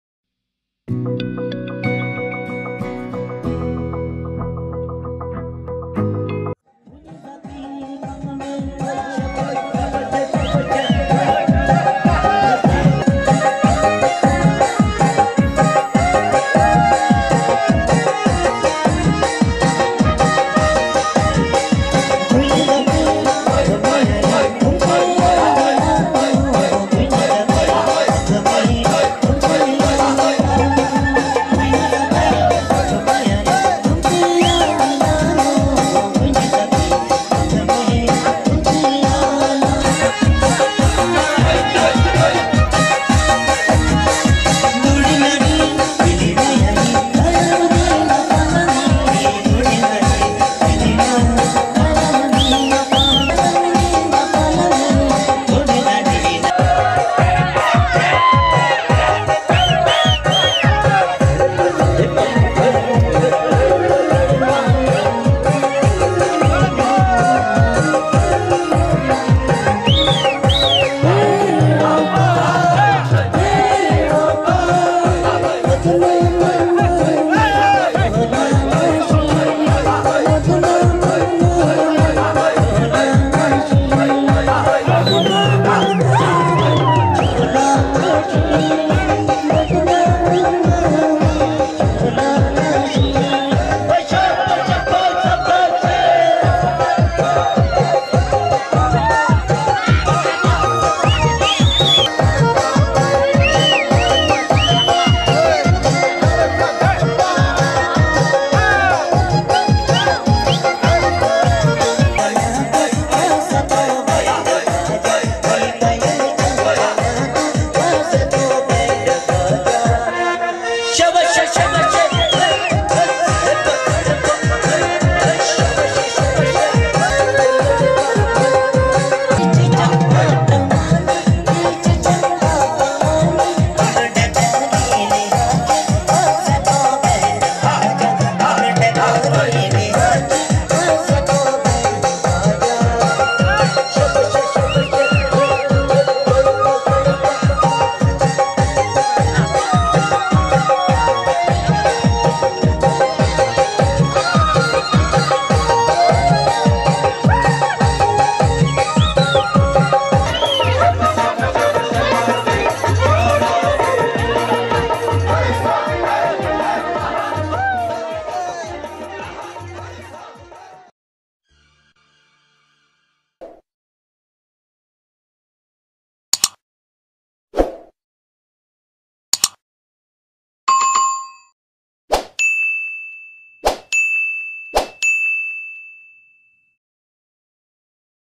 Beautiful Kinnauri Dance II Old Moment II Wedding Dance 2014 II Himachali Wedding
Video BY:- Raj Vimal.